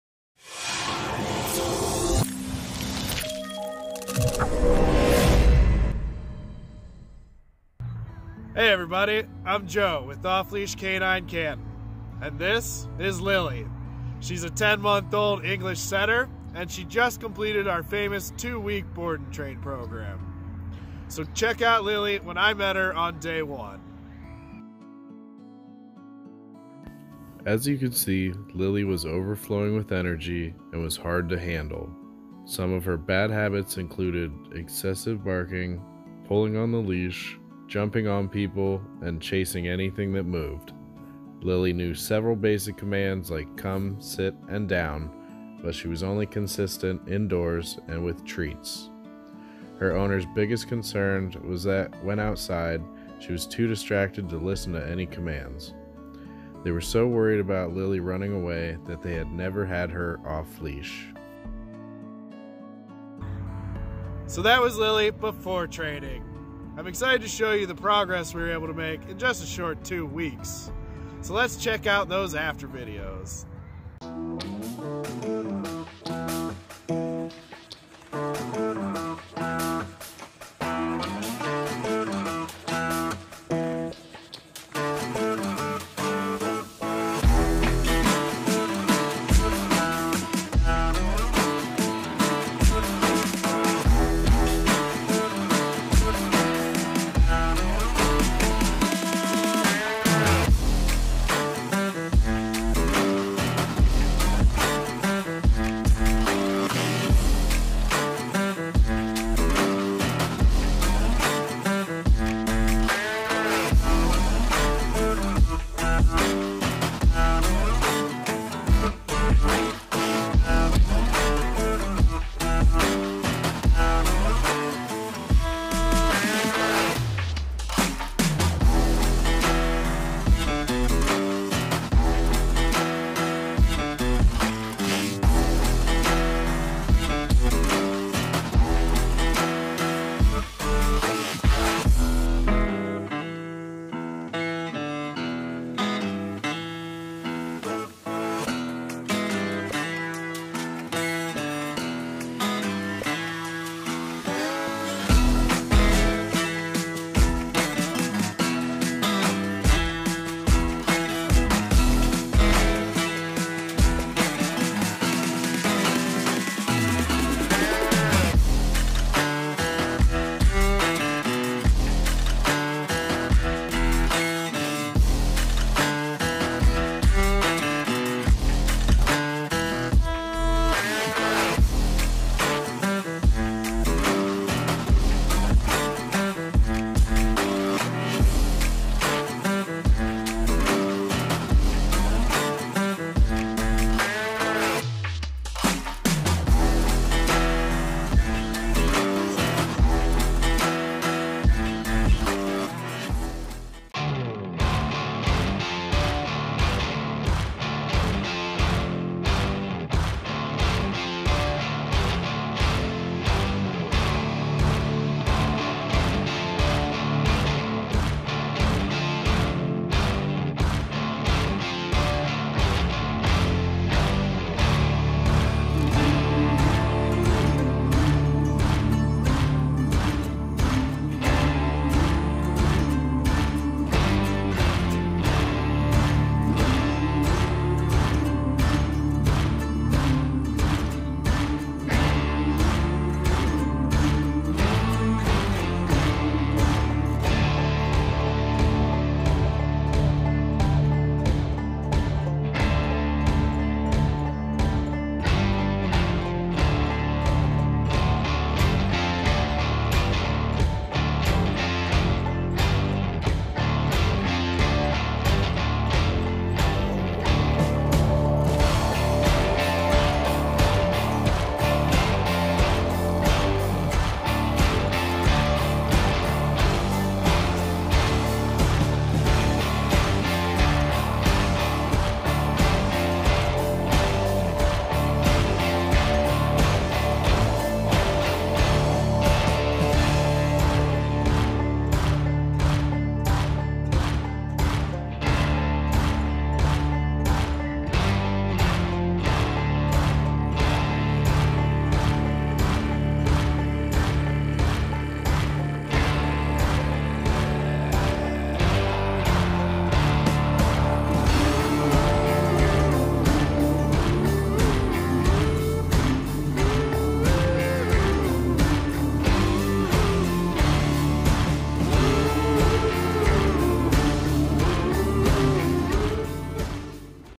Lily | 10 Month Old English Setter | Off Leash Obedience | E-Collar Training
Take a look at the progress we were able to make with Lily in just 2 weeks!